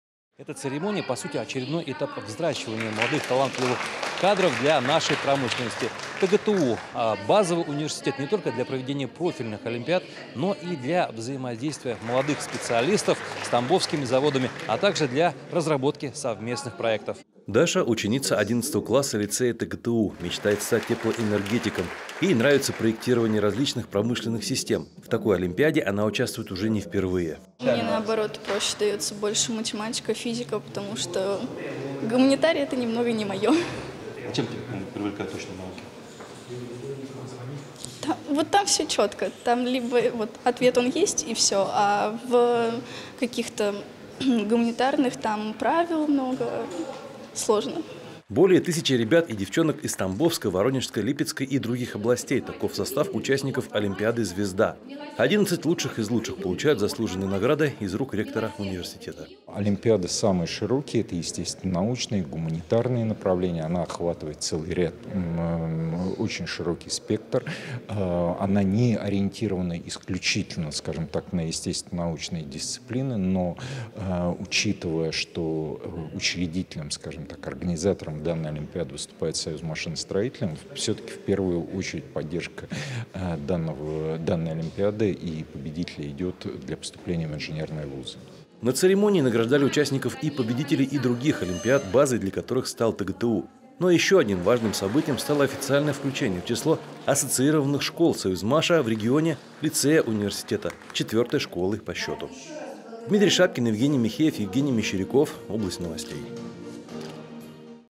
В ТГТУ наградили победителей всероссийской олимпиады «Звезда»
11 призеров и более тысячи участников со всей страны. В ТГТУ наградили победителей всероссийской олимпиады «Звезда».

Более тысячи ребят и девчонок из Тамбовской, Воронежской, Липецкой и других областей - таков состав участников олимпиады «Звезда». 11 лучших из лучших получают заслуженные награды из рук ректора университета.